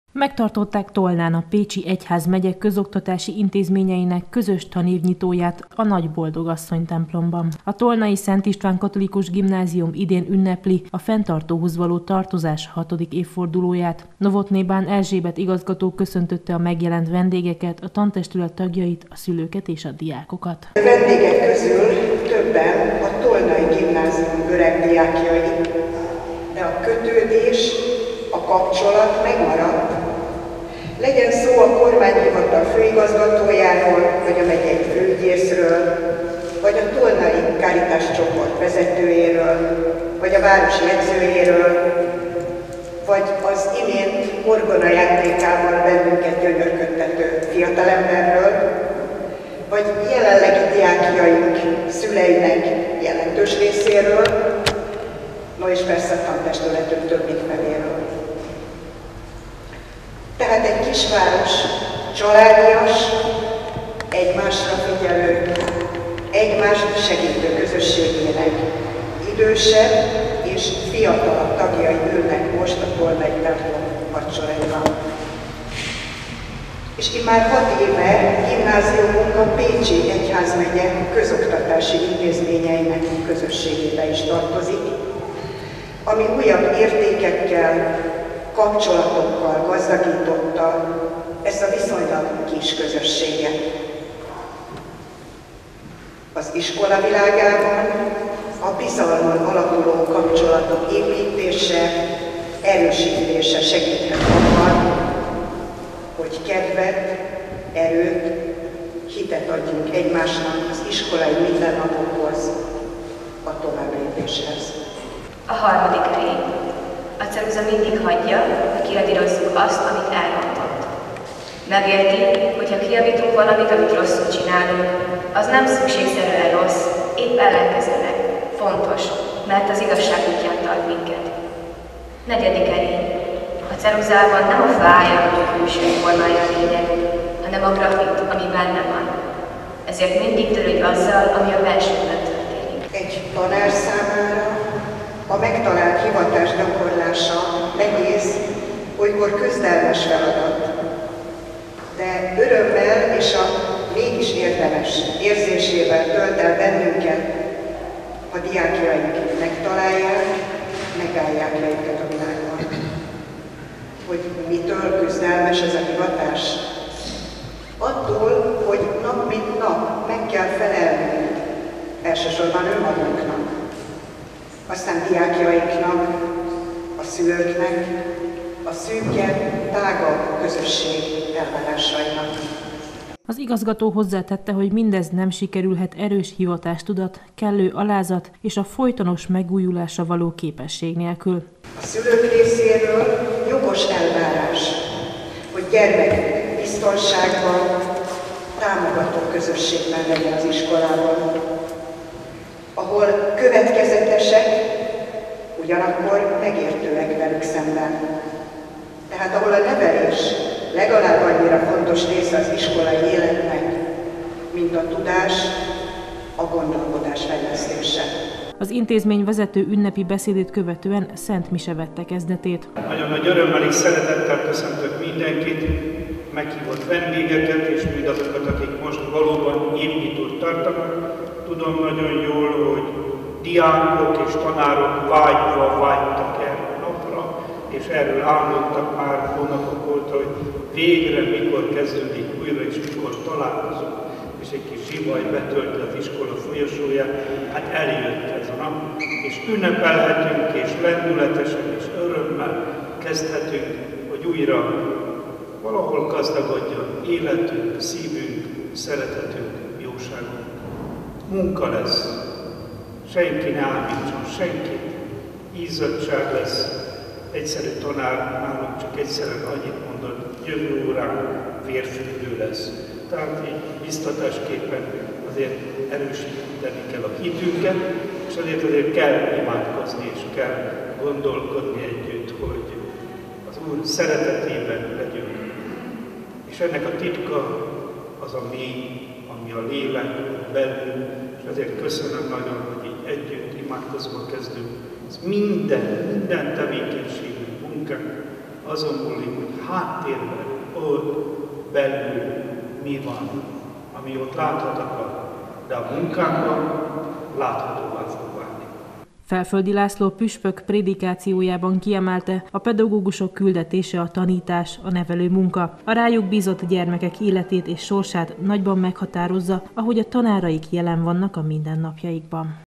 Együtt ünnepelt a Pécsi Egyházmegye közoktatási intézményeinek közössége
Tolnán tartották a Pécsi Egyházmegye közoktatási intézményeinek tanévnyitóját. Az eseményen Novothné Bán Erzsébet igazgató mondott ünnepi beszédet. Ezt követően pedig püspöki szentmisén vehettek részt a diákok és a pedagógusok.